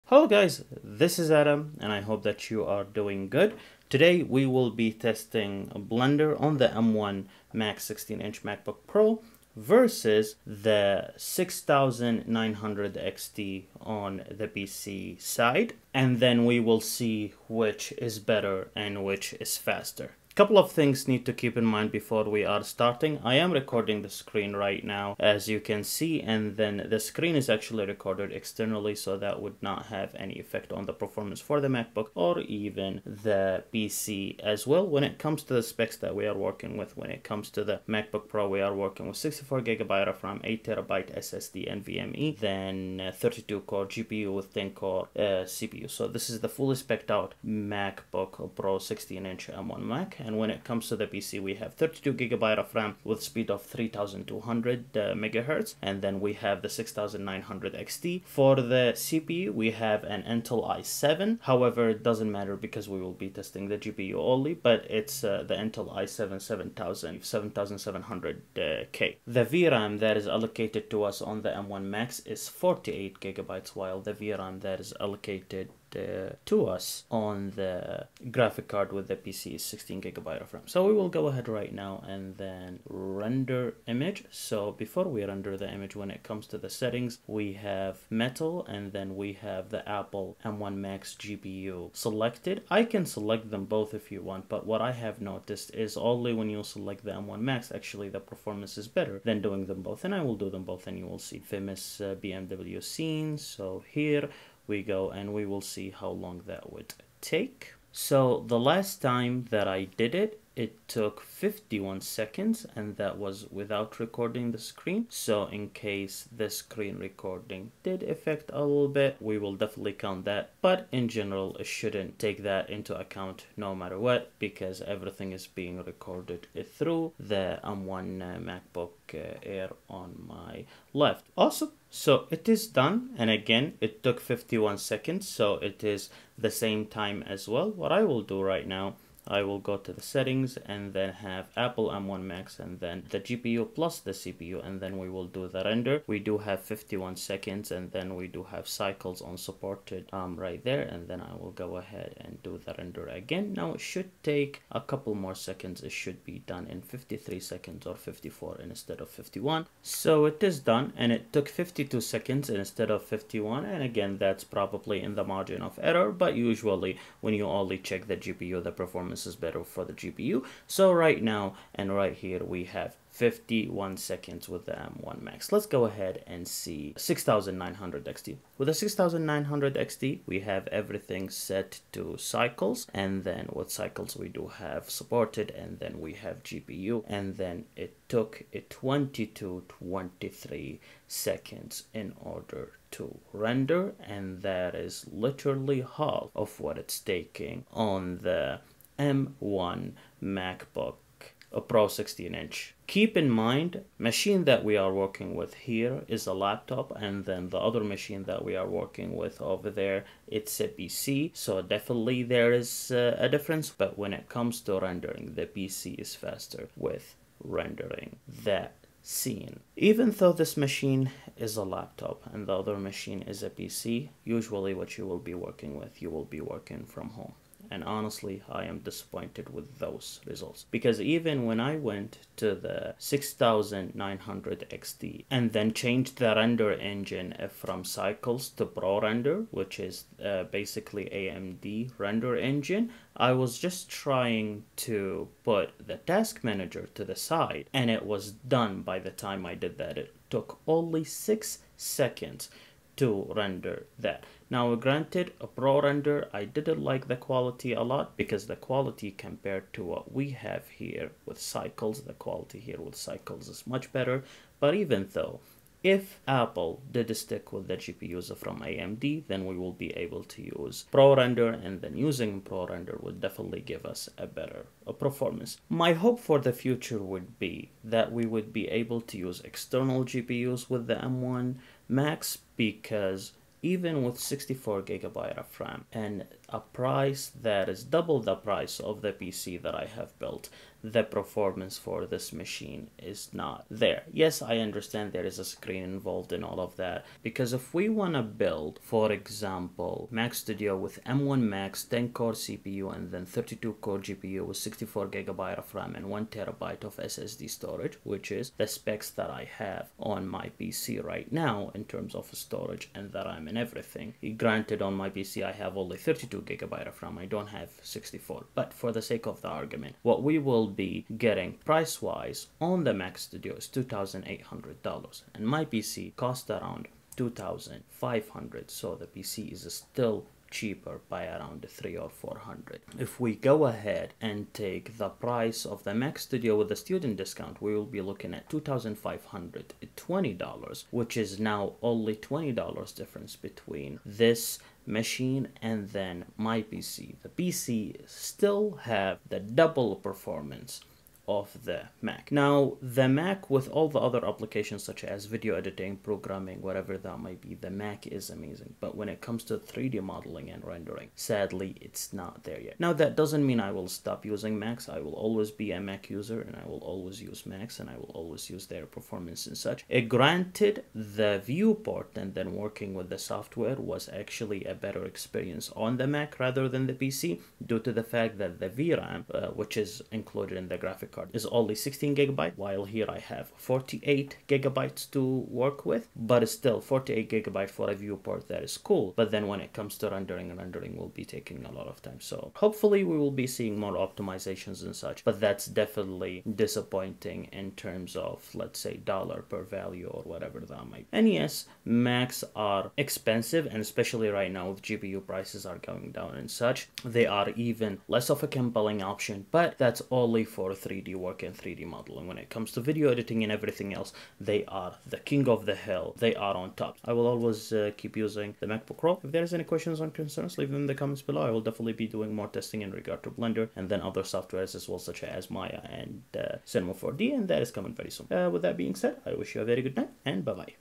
M1 MAX vs RX 6900 XT For 3D Rendering
PC vs M1 Max head to head in 3D rendering, which is better?